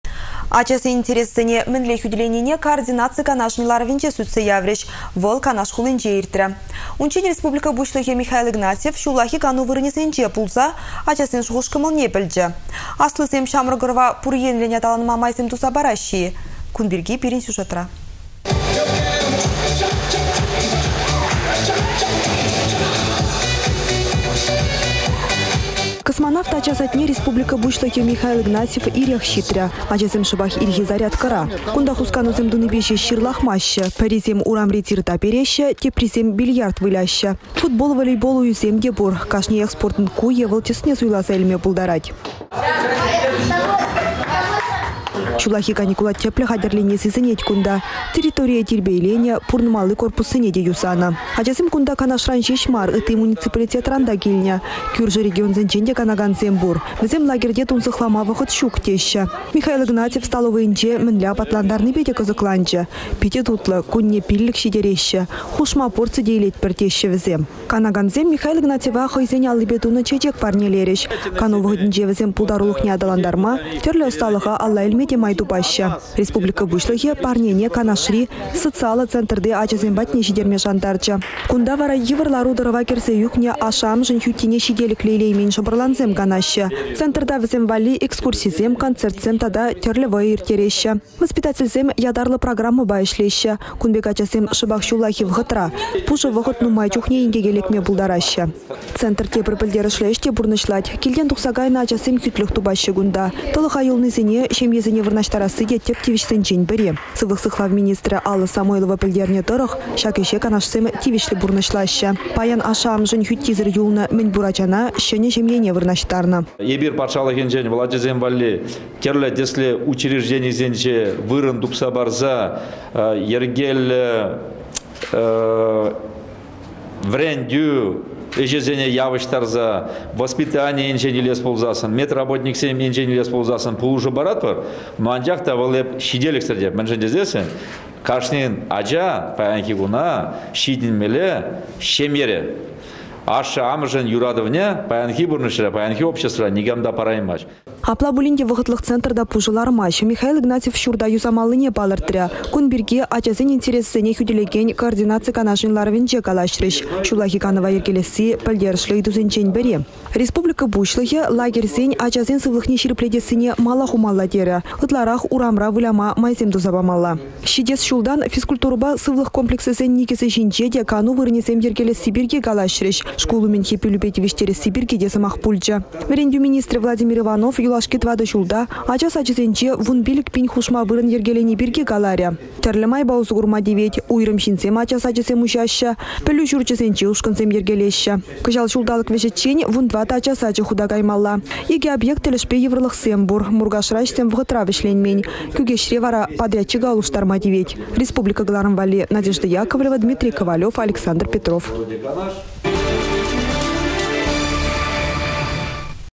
«Михаил Игнатьев кану вырăнĕсенче пулса курчĕ»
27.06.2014
Ачасен интересĕсене мĕнле хÿтĕленине координаци канашĕн ларăвĕнче сÿтсе яврĕç. Вăл Канаш хулинче иртрĕ. Унччен Республика пуçлăхĕ Михаил Игнатьев çуллахи кану вырăнĕсенче пулса ачасен шухăш-кăмăлне пĕлчĕ. Аслисемпе çамрăк ăрăва пур енлĕн аталанма майсем туса параççĕ-и, кун пирки пирĕн сюжетра.